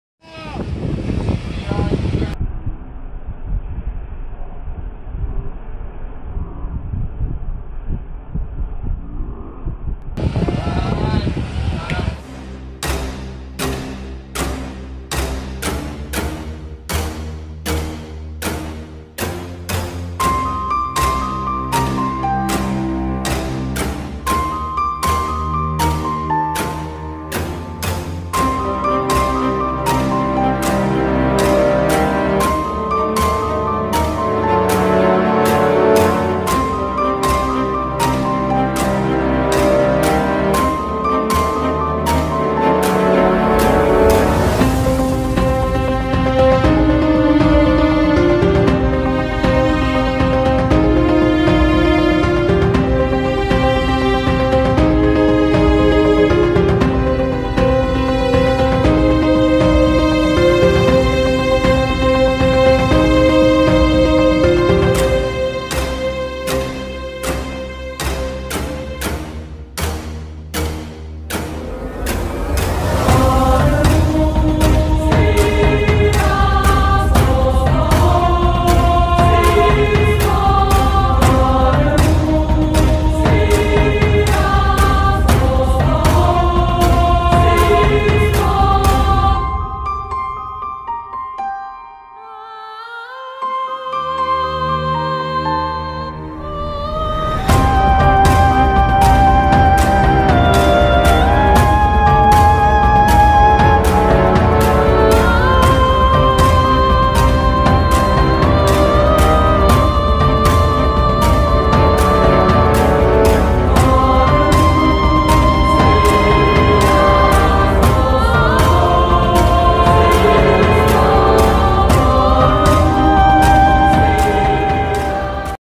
Cabarete | Master of the Ocean 2021
Short clip of this year's Master of the Ocean competition in Cabarete, Dominican Republic.